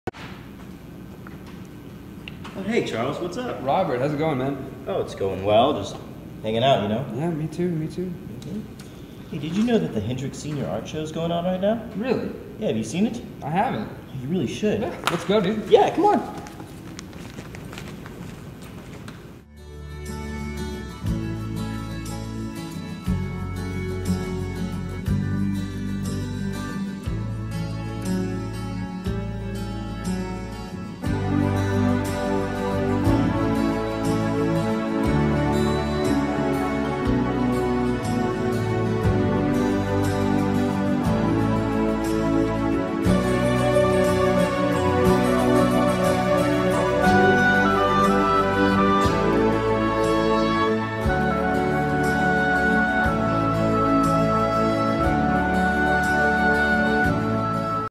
Senior Art Show 2012 Trailer
The Hendrix Senior Art Exhibition opening reception is Thursday, April 5, 2012 from 6 to 8 p.m. in Trieschmann Gallery. Featured artists include Courtney Johnson, Sabrina Moore, Sam Hoiby, Frances Bowen, Allie Thompson, Anna Hunter, Nicole Morgan, Nora Jenks, and Will Lentz. The exhibition runs through May 11.